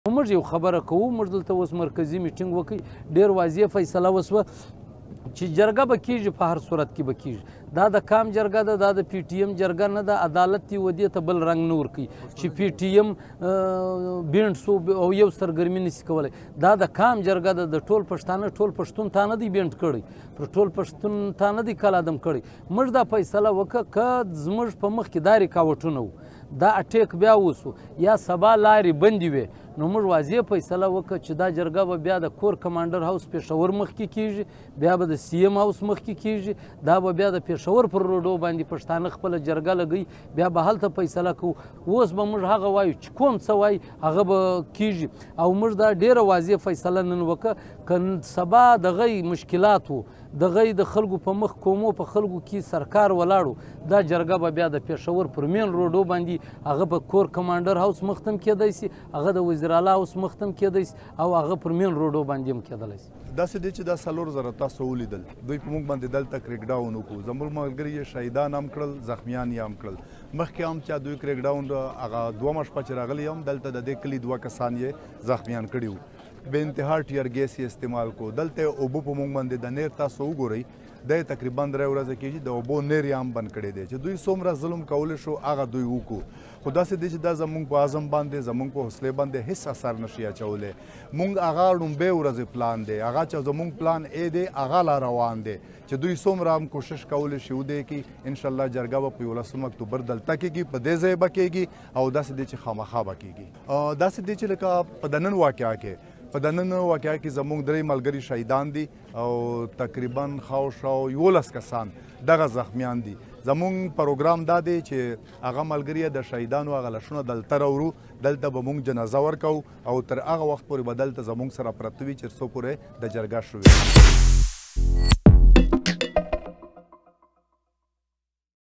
ويډيو: د پښتون ژغورنې غورځنګ مشران د خپل راتلونکي پلان په اړه غږېږي
د پښتون ژغورنې غورځنګ (پي ټي اېم) مخکښ مشران عبدالصمد خان او نور الله ترين وايي په هر حالت کې به په ۱۱ م اکتوبر جرګه کوي. دوي وايي که حکومت په خيبر کې جرګې ته پرېنښودل نو بيا دغه جرګه د پېښور په سړکونو کوي.دا په داسې کې ده چې پي ټي اېم وايي د خیبر ضلعې په رېګي للمه کې د "پولیسو په مستقیمو ډزو کې" یې درې غړي وژل شوي دي.
په همدې حال کې د پاکستان کورنيوو چارو وزیر محسن نقوي د اکتوبر پر نهمه په خبري غونډه کې تور پورې کړ چې پي ټي اېم يې د ریاست پر ضد د فعاليت له وجې ناقانونه اعلان کړی او د جلسو کولو اجازه نه لري. بلخوا د غورځنګ مشر منظور پشتين ويلي چې خپلې سوله ییزې مبارزې ته دوام ورکوي.